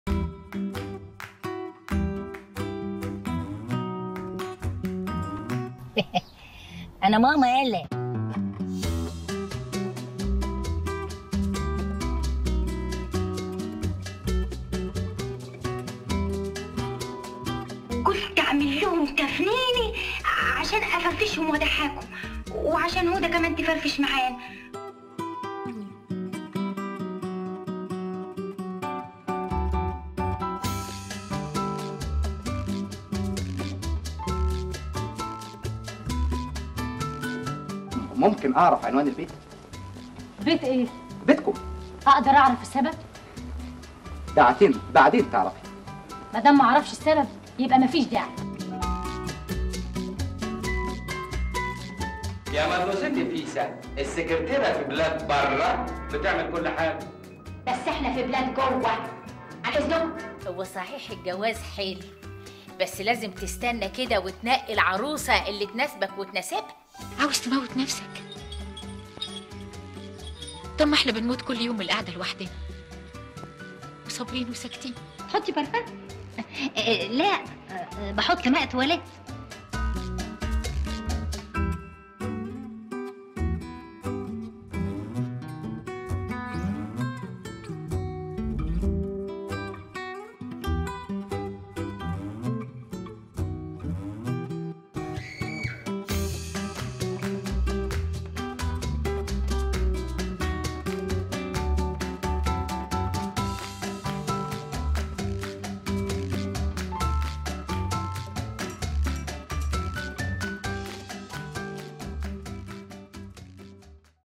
إنعام سالوسة.. قادرة على كل دور
"أنا ماما يالا".. مبدعة في كل أدوارها على الرغم من إنها لم تنل ما تستحق من التكريم

إنعام سالوسة المتألقة دائمًا